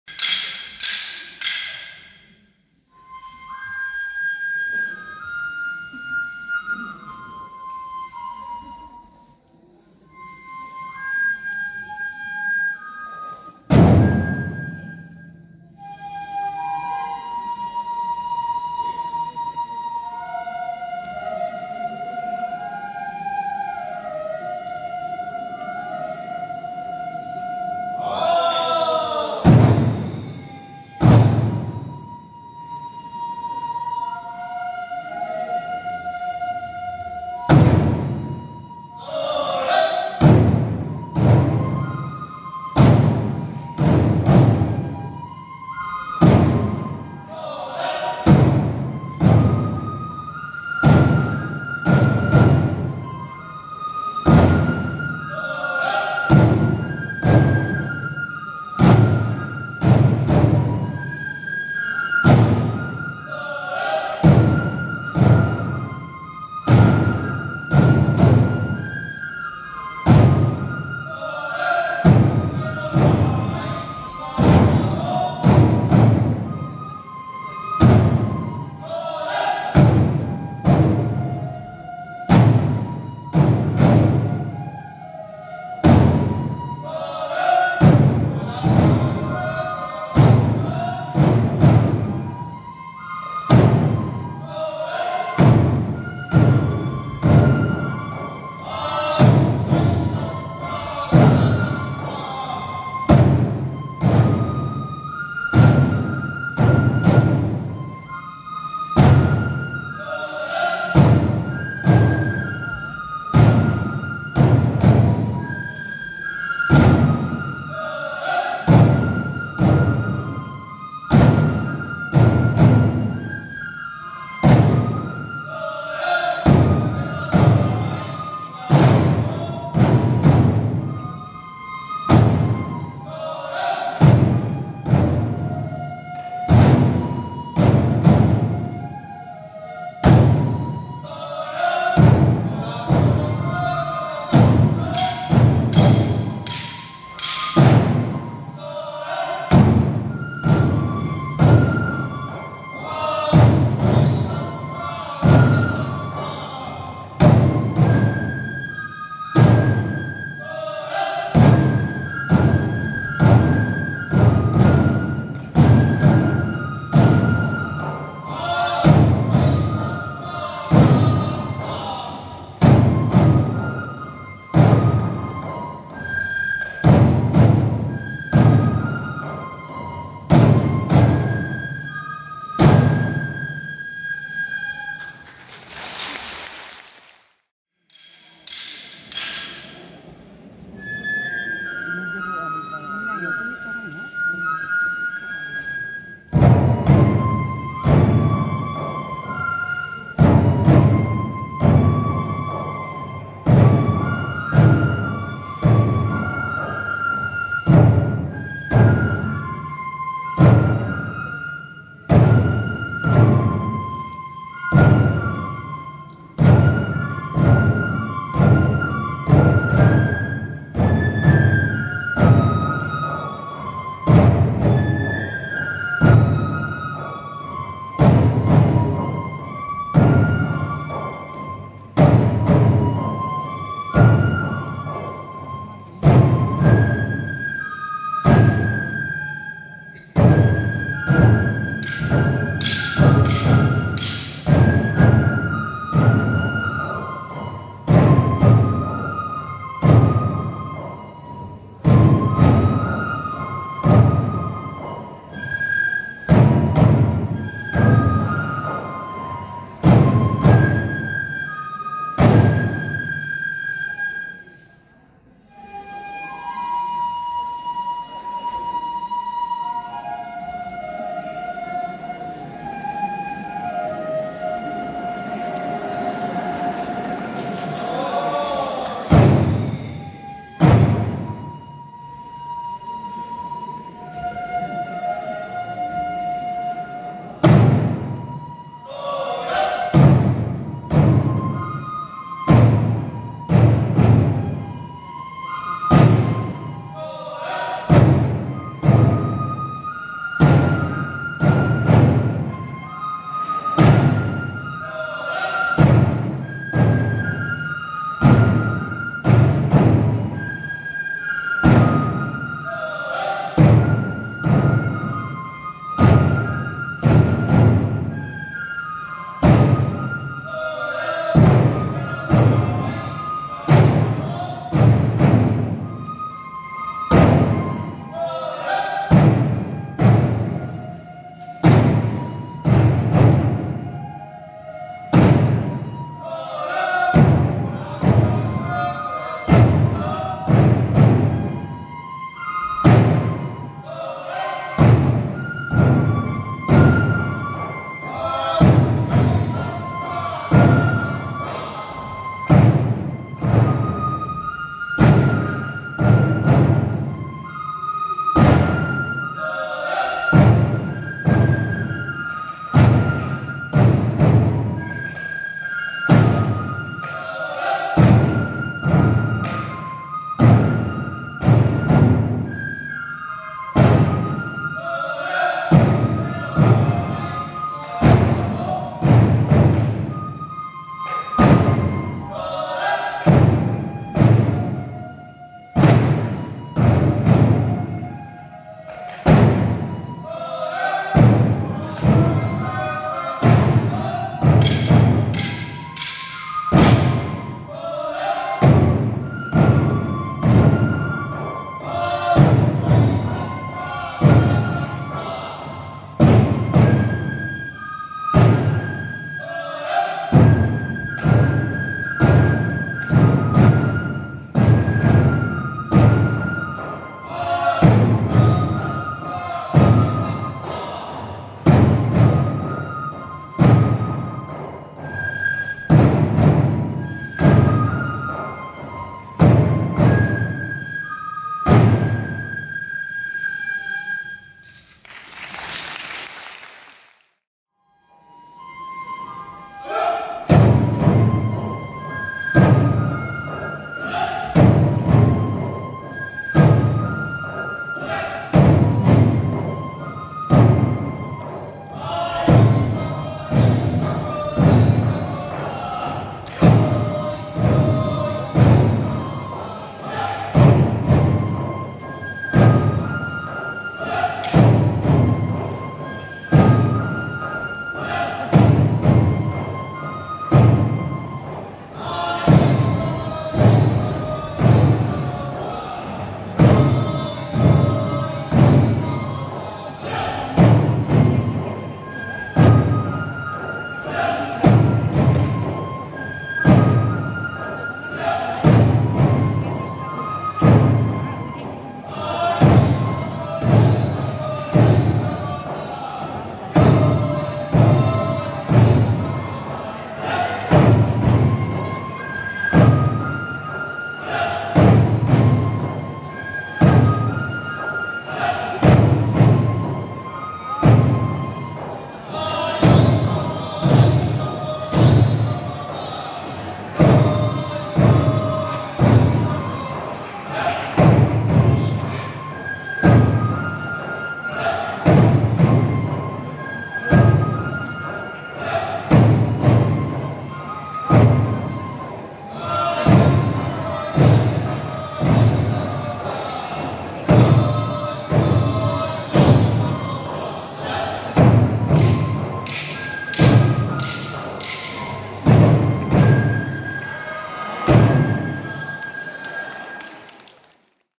Hatsuuma Matsuri Tigertanz Teil 1 in Kami-machi, Miyagi, JAPAN - April 2024 (宮城県加美町)
In diesem Video könnt ihr einen Eindruck vom japanisch Tiegertanz: Toramai (虎舞 oder とらまい) bekomen. Dieser wurde in der Bach Hall im Rahmen des Hatsuuma Matsuri in Kami-machi, Miyagi, Japan (宮城県加美町) im April 2024 aufgeführt. Das eigentliche Festival findet jährlich am 29. April statt.

Anmerkung: Das Video nutzt bewusst den originalen Ton und ist wenig bearbeitet, damit es sich für dich so anfühlt, als wärst du selbst dabei.